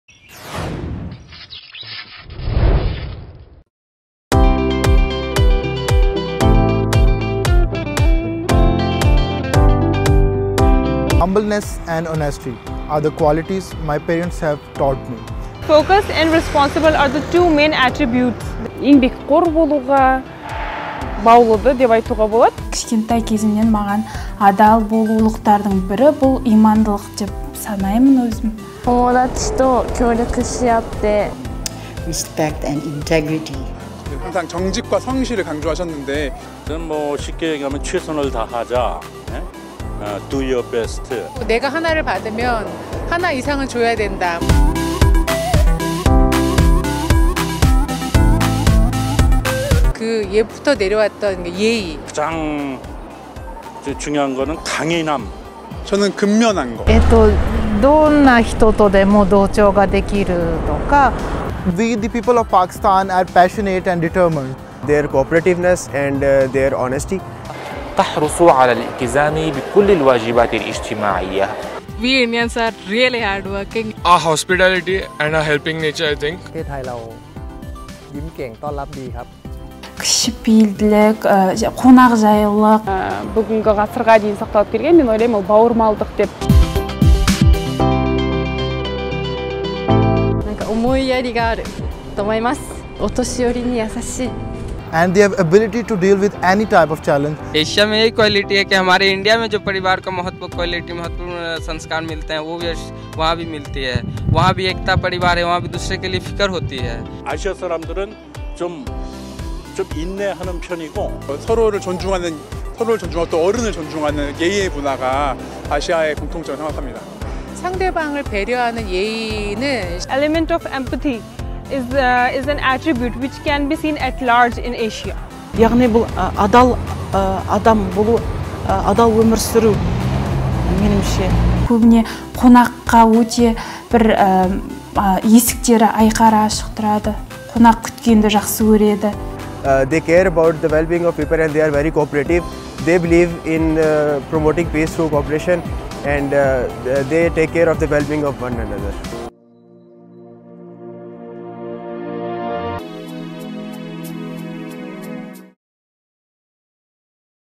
The "secret code" of Asia
What quality do Asian people cherish the most, and what belief do they share? Here is the "secret code" of the largest continent on Earth.